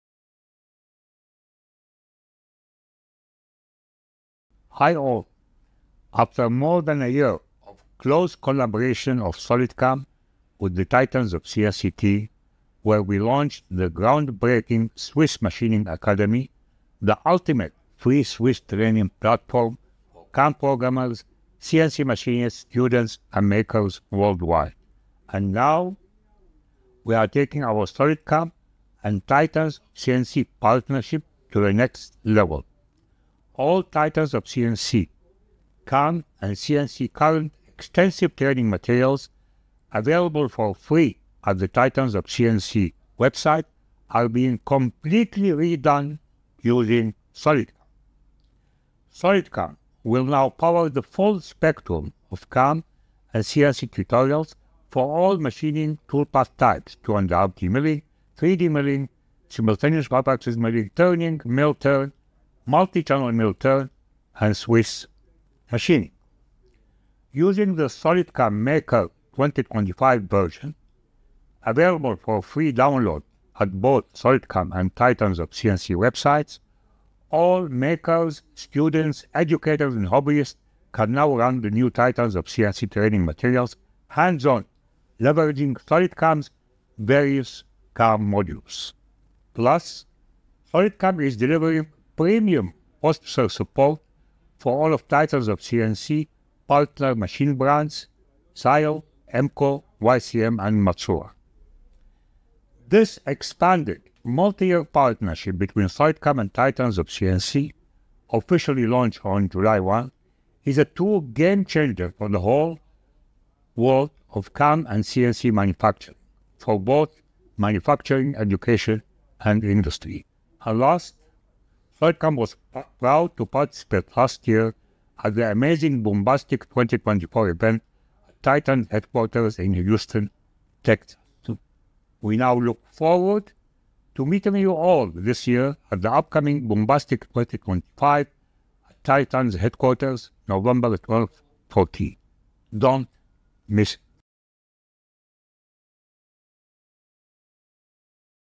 SolidCAM x Titans of CNC: Next level Game-Changing Partnership!
The Titans of CNC & SolidCAM Partnership just moved to the next highest level! 🚀

All Titans of CNC CAM and CNC training materials, including 2.5D Mill, 3D Mill, Sim5X, Mill-Turn, Multi-Channel Mill-Turn, and Swiss Machining, will now be redone for SolidCAM.

Run it all with SolidCAM Maker 2025, FREE for all Makers, Students, Educators & Hobbyists!

✅ Premium SolidCAM post processors for Syil, Emco, YCM & Matsuura.

📍 See you at Boombastic 2025 – Nov 12–14 @ Titans HQ!